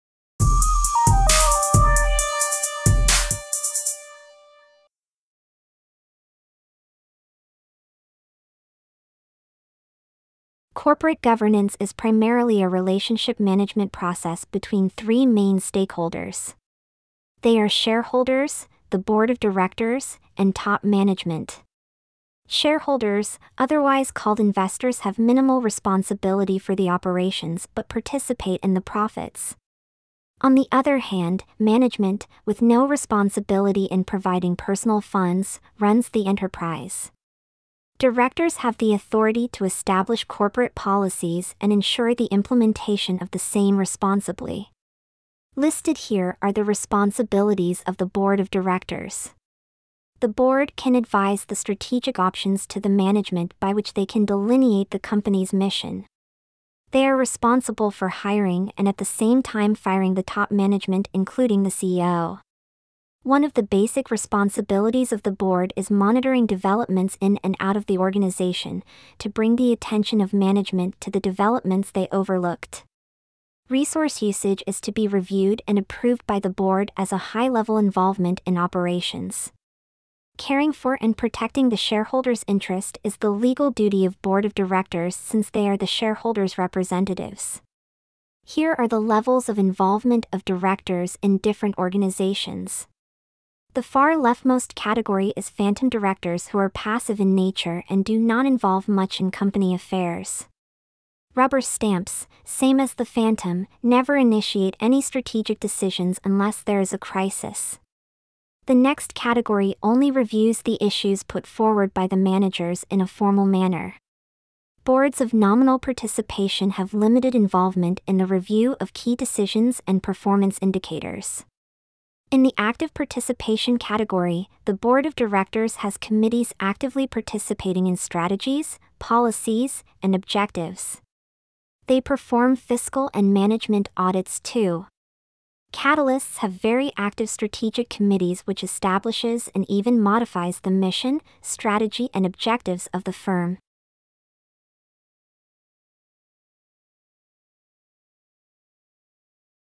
Strategic Management - Corporate Governance (STR-B1.4)
Basics of corporate management in a strategic management perspective